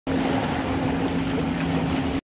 20200221 single dolphin
Single bottlenose dolphin in Algoa Bay